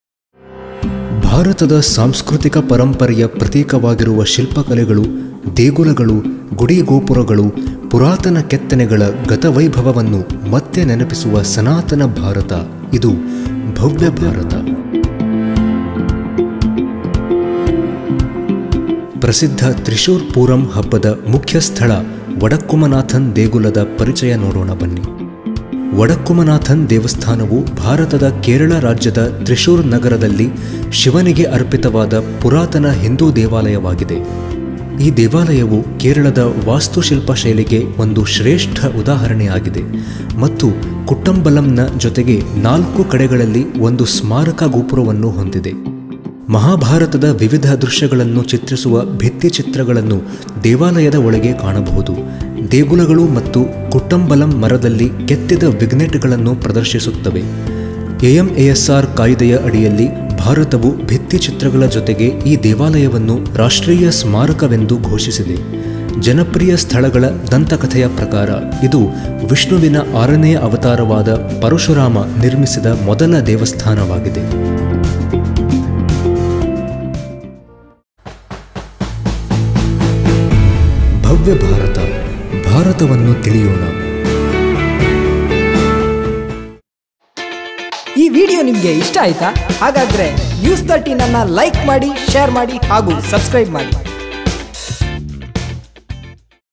ಭವ್ಯ ಭಾರತ Episode 3 - ವಡಕ್ಕುಮನಾಥನ್ ದೇಗುಲ | News13 | Right Angle Media - RAM
ಭವ್ಯ ಭಾರತ’ದ ಸರಣಿಯಲ್ಲಿ ಇಂದು ’ವಡಕ್ಕುಮನಾಥನ್ ದೇಗುಲ‌’ದ ಪರಿಚಯ ಮಾಡಿಕೊಳ್ಳೋಣ.

ಭಾರತದ ಸಾಂಸ್ಕೃತಿಕ ಪರಂಪರೆಯ ಪ್ರತೀಕವಾಗಿರುವ ಶಿಲ್ಪಕಲೆಗಳು,  ದೇಗುಲಗಳು, ಗುಡಿಗೋಪುರಗಳು, ಪುರಾತನ ಕೆತ್ತನೆಗಳ ಗತವೈಭವವನ್ನು ಮತ್ತೆ ನೆನಪಿಸುವ ಸನಾತನ ಭಾರತ, ಇದು "ಭವ್ಯ ಭಾರತ". ಭಾರತವನ್ನು ತಿಳಿಯೋಣ.

ಉತ್ತಮ ಸಂಗತಿಗಳನ್ನು ತಿಳಿಯೋಣ, ಸಮಾಜಕ್ಕೆ ತಿಳಿಸೋಣ.